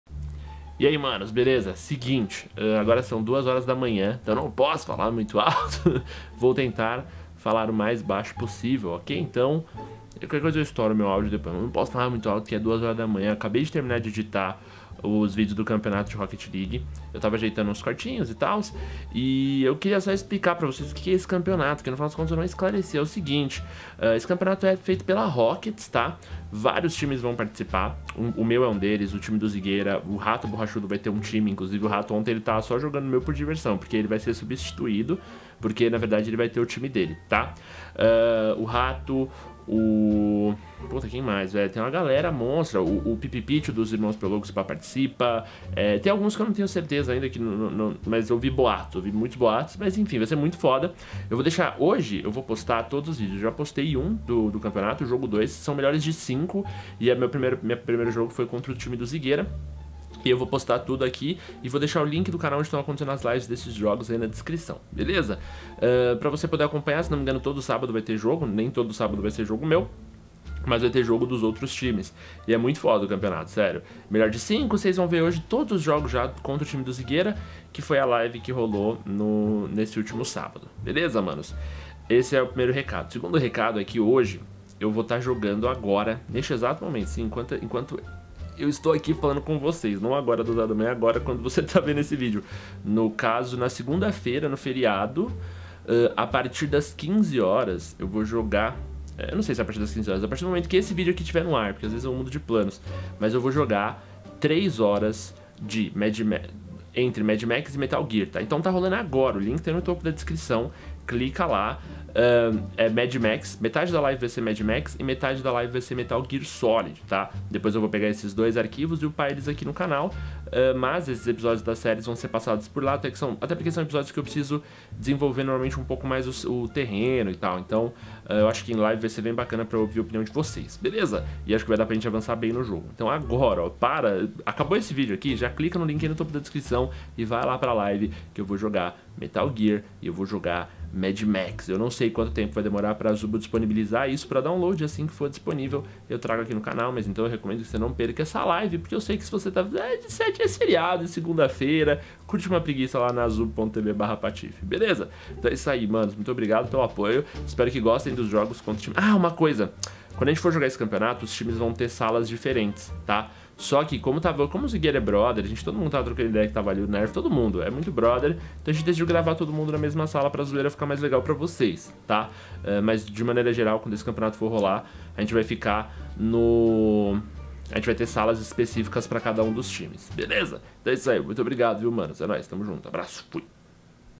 CAMPEONATO DE ROCKET LEAGUE, MAD MAX E METAL GEAR SOLID
Vim explicar o campeonato de Rocket League e anunciar minha live :)

Valeeu!